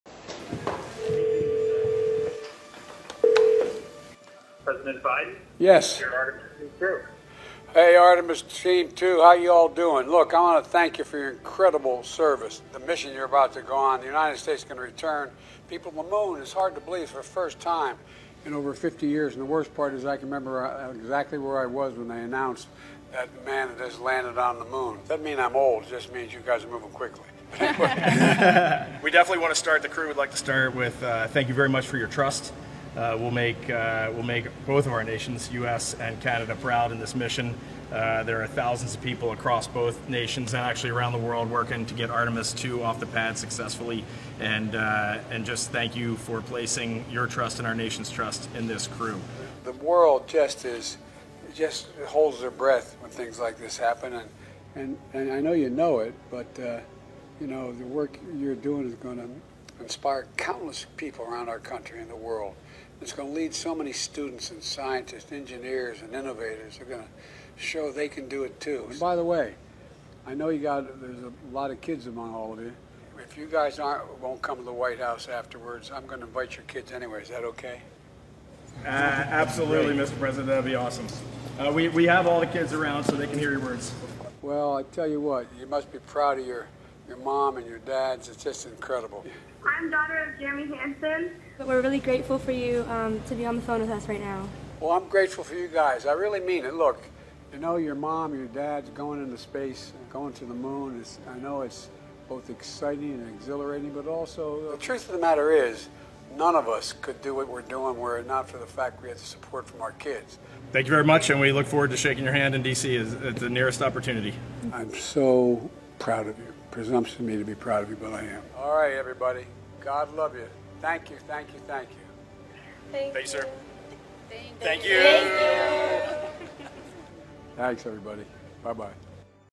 President Biden Calls Artemis 2 Crew
LAUNCH TIMELINE
00:00:00 - Start of Stream

VIDEO SOURCES:
White House